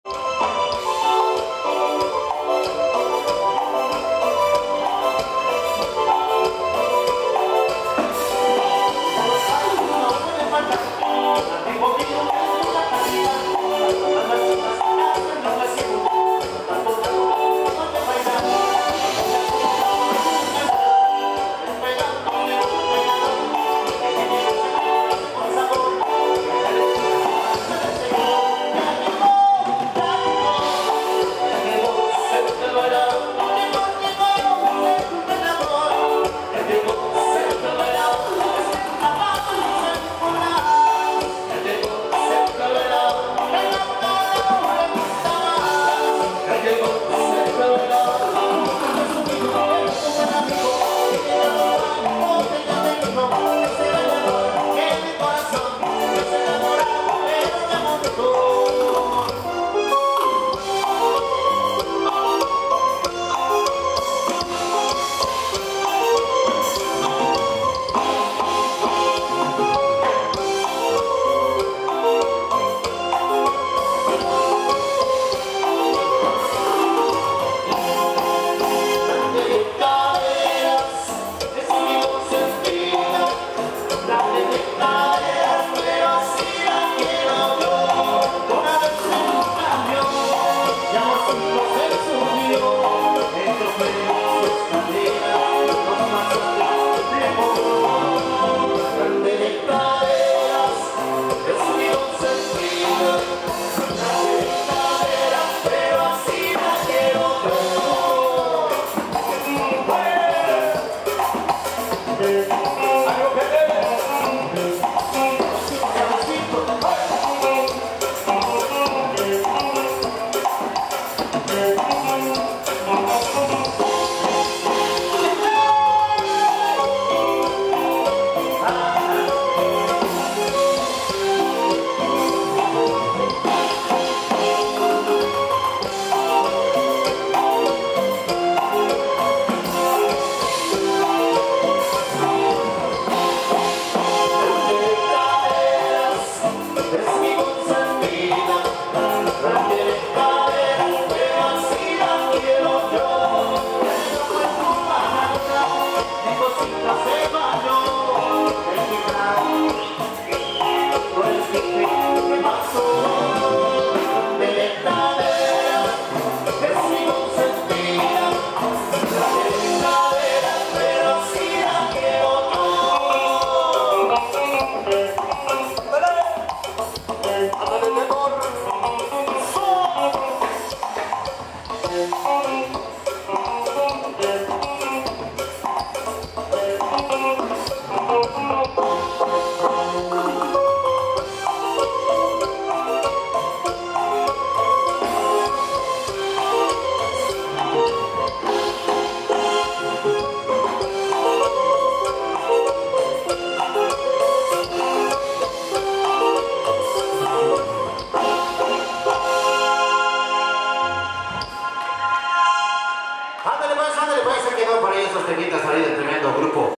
La gente baile y baile con Los Dominguez en Barranco Blanco
POPURRÍ DE BRONCO EN VIVO CON LOS DOMINGUEZ!!!